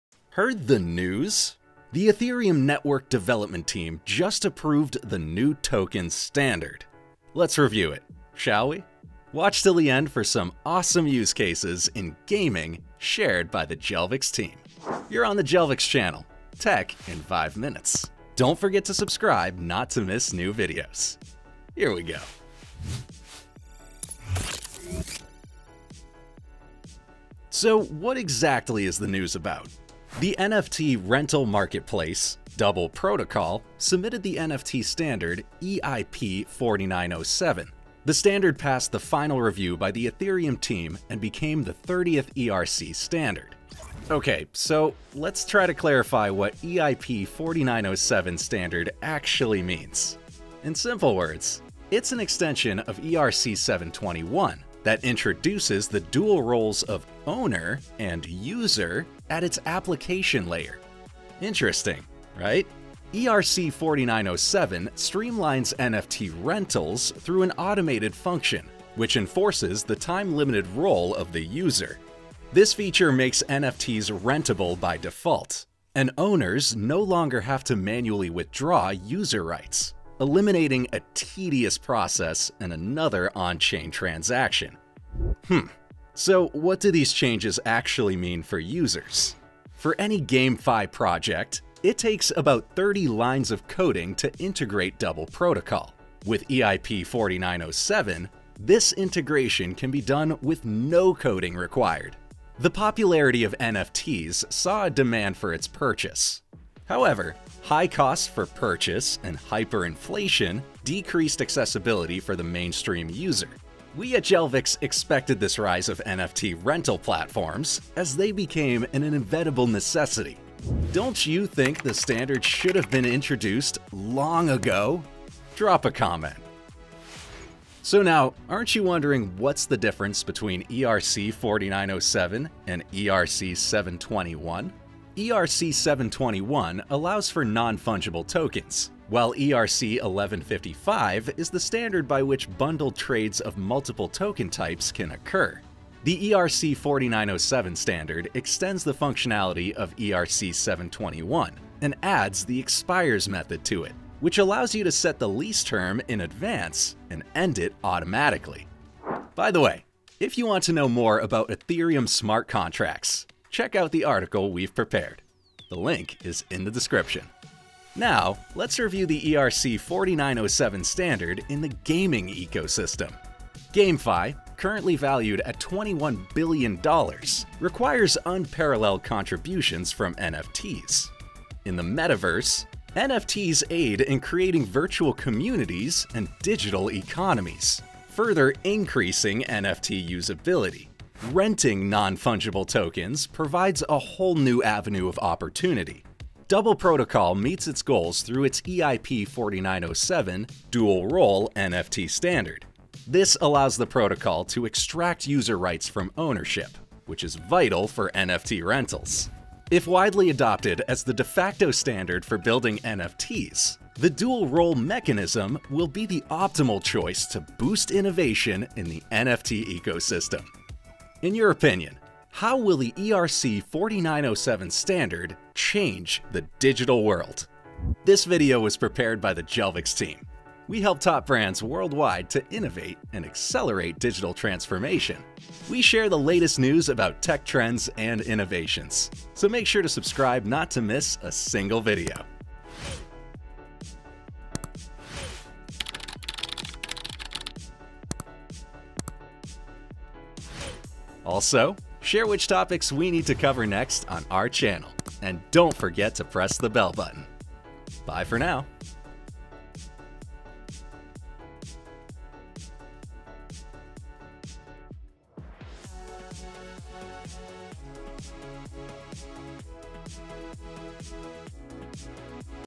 Boosting Efficiency, Reducing Fees: The advantages of the new token standard...
The Ethereum network development team approved the new token standard - EIP-4907! Let’s review it.

▶ Time codes:
00:00 What is EIP-4907?
02:08 ERC-4907 vs ERC-721
02:48 ERC-4907 standard used in gaming
03:48 Contact Jelvix

▶ About this video:
ERC-4907 streamlines NFT rentals through an automated function, which enforces the time-limited role of the user. This feature makes NFTs rentable by default. And owners no longer have to manually withdraw user rights, eliminating a tedious process and another on-chain transaction.